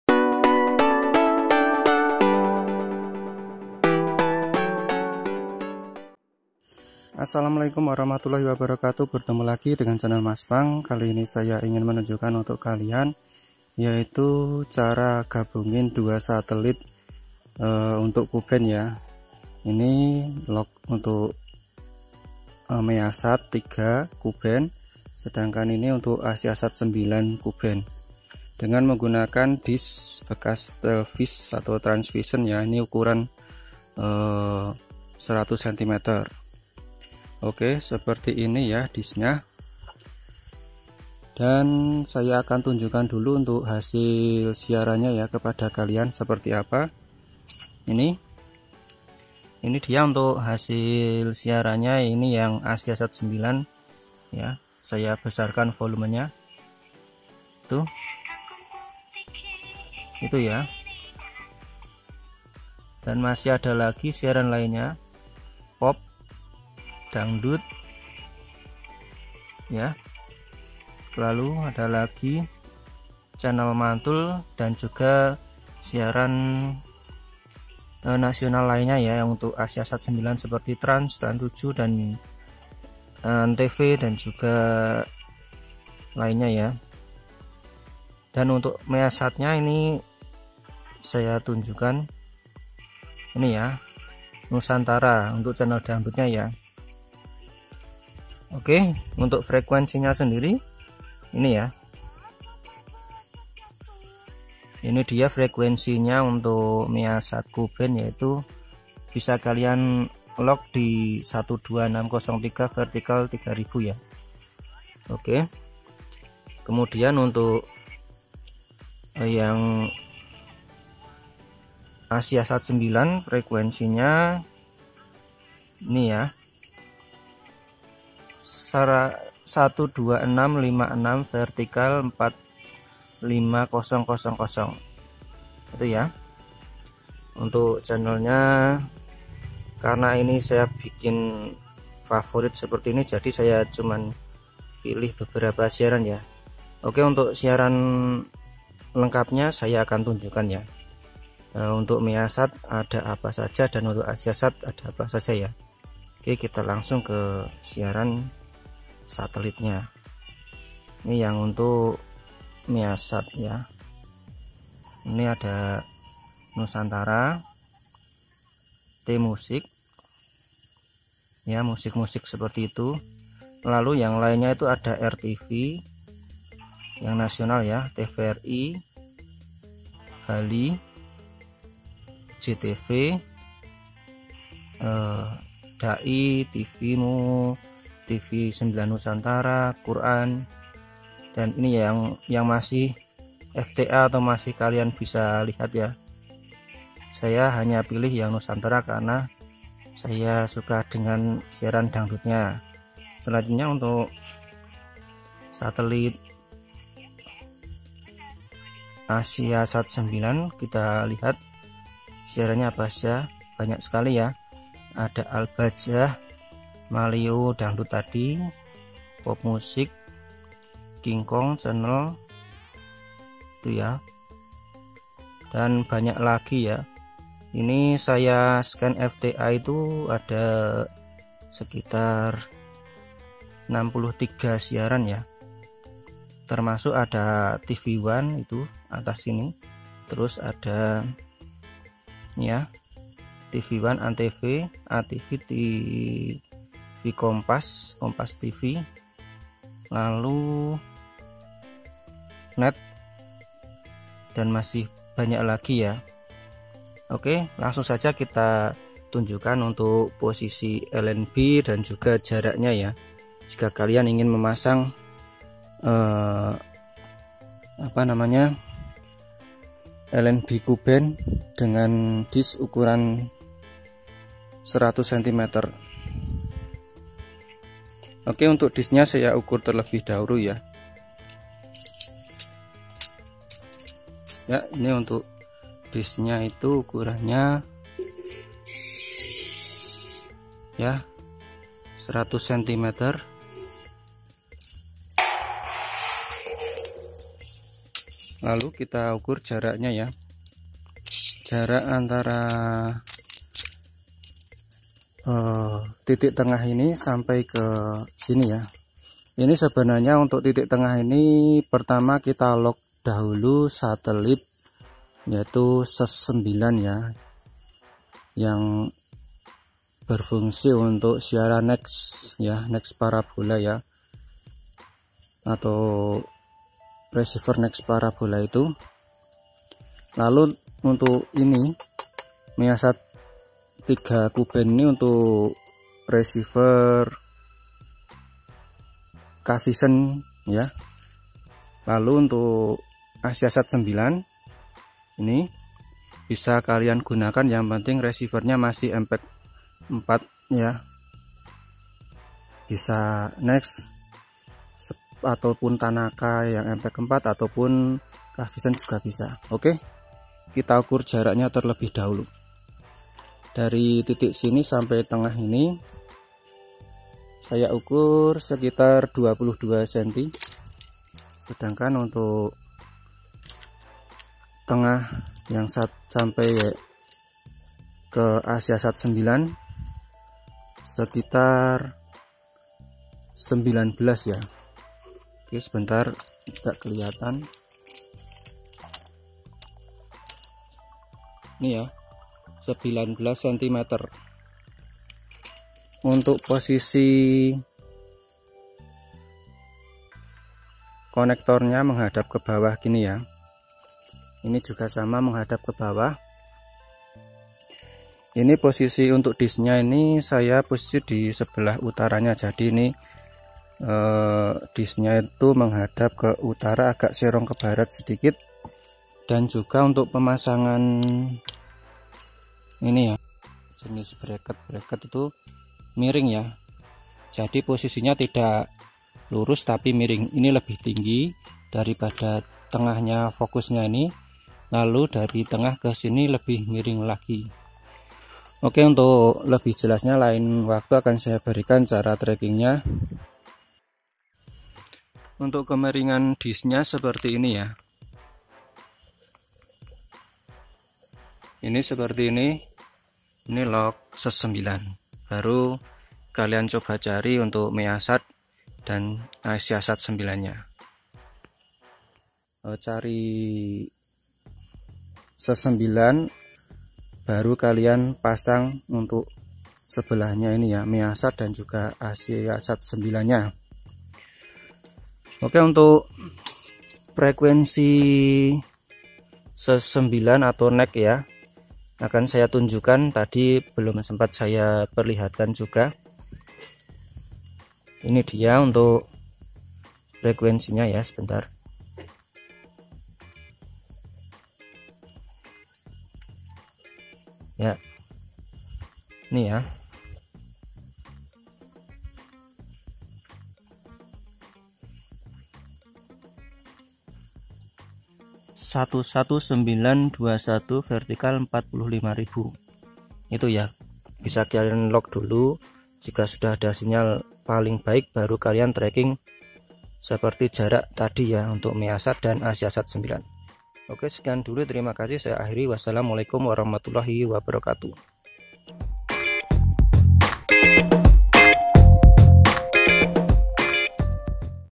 MUDAHH!! CARA GABUNGIN TIGA SATELIT MEASAT 3, SES 9, DAN ASIASAT 9 KU BAND SATU DISHH!!
Berikut ini cara lock satelit measat 3 ku band, ses ku band dan asiasat 9 ku band digabung menjadi satu dish.
Untuk posisi arah dish dan kemiringan beda tempat berbeda ya sesuai dengan tkp kalian ini saya posisi di jawa tengah.
Yang tentunya bagi kalian pecinta siaran dangdut maleo, channel dangdut, channel mantul, madu tv dan nusantara yang bisa kalian nikmati menggunakan reseiver mpeg 4.
Dengan menggunakan dish bekas transvision ataupun telkomvision ukuran 100cm dengan tambahan besi berlubang atau braket untuk memasang lnbnya.
Dan untuk belinya bisa dibengkel sepeda motor karena besi tersebut berfungsi untuk variasi penyangga kenalpot sepeda motor.
Semoga bermanfaat.